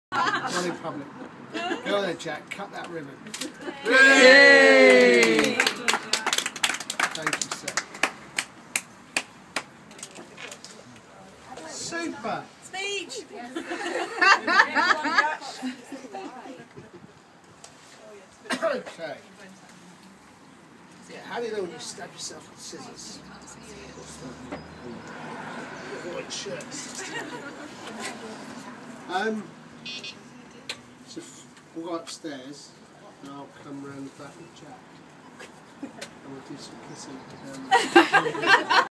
Hollowgate House Clinic Open Day (Liberty Physiotherapy)
Hollowgate House is open, open, open...Officially.
The legendary Jack Mitchell (Olympic Torchbearer Ackworth 2012) opens the clinic you guys! For the afternoon 'informal' opening. Officially the Lord Mayor and Lady Mayoress of Rotherham opened the clinic in the morning. They were great. Everyone was great. It was so good to see everyone who showed up. A big shout out to Jack and his family. Y'all made the day ;)